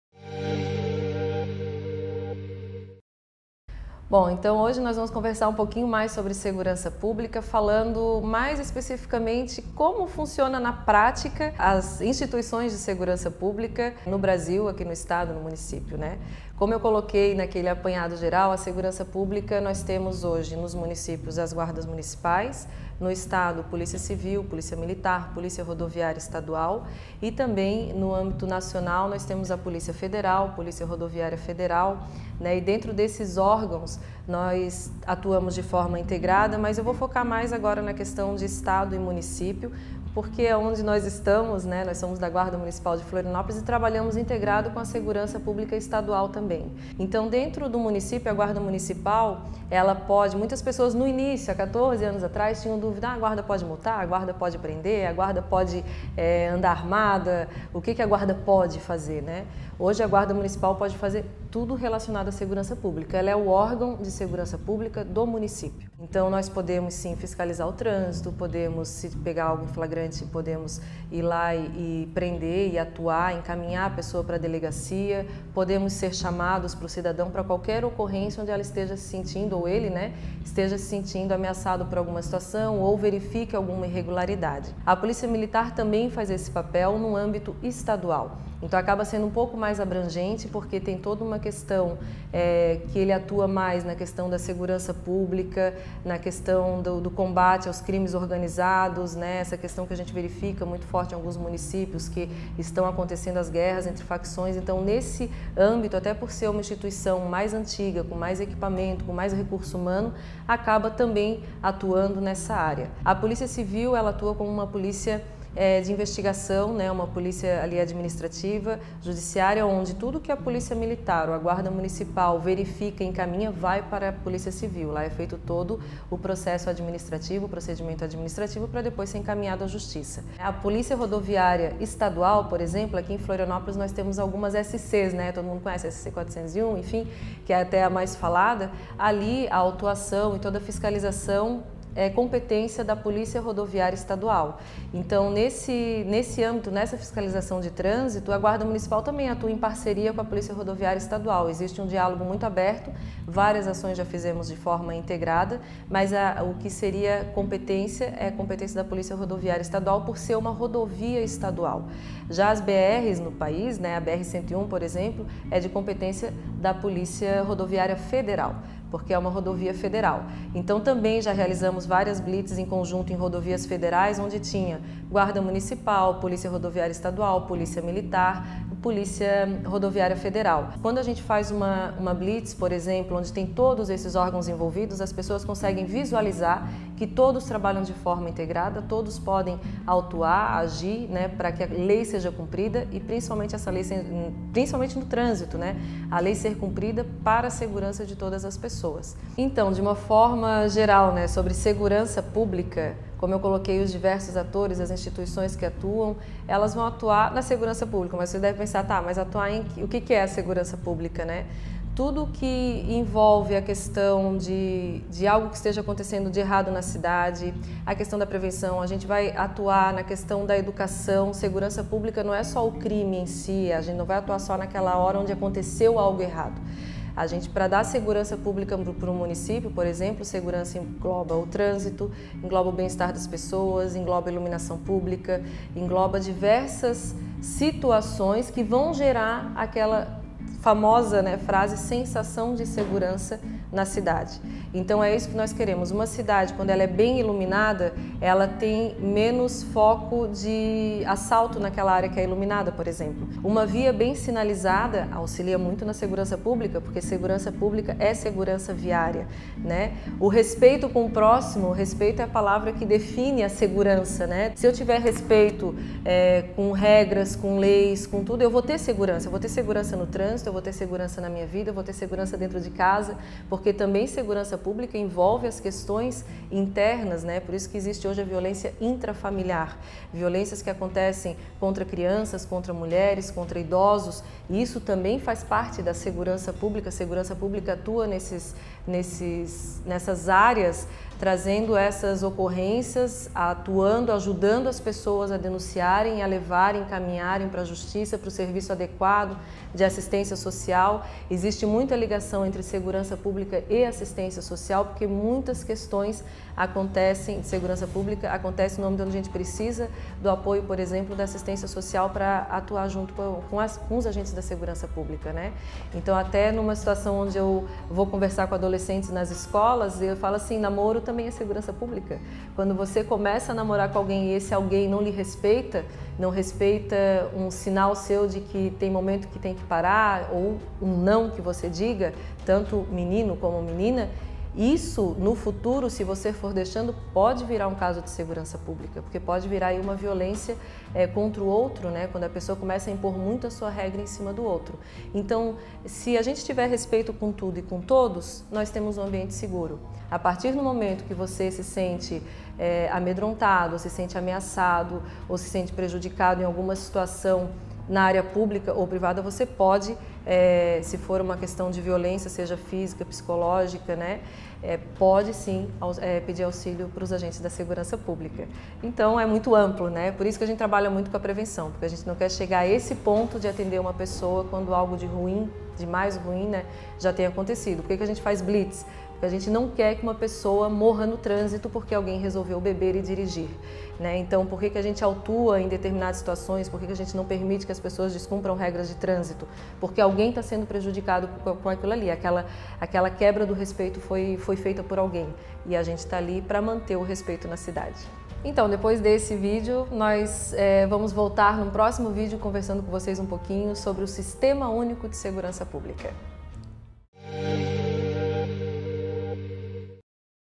Segurança Pública Municipal | Diferenças entre Guarda Municipal, Polícia Civil, Militar e Rodoviária
No segundo vídeo da sequência, Maryanne Mattos explica o que é segurança pública e de que forma os agentes de fiscalização atuam para garantir a segurança nas cidades.

Para se aprofundar mais no tema Segurança Pública no Brasil, não deixe de acessar:

Para ficar por dentro de TODOS os órgãos de Segurança Pública do Brasil, confira este texto do nosso portal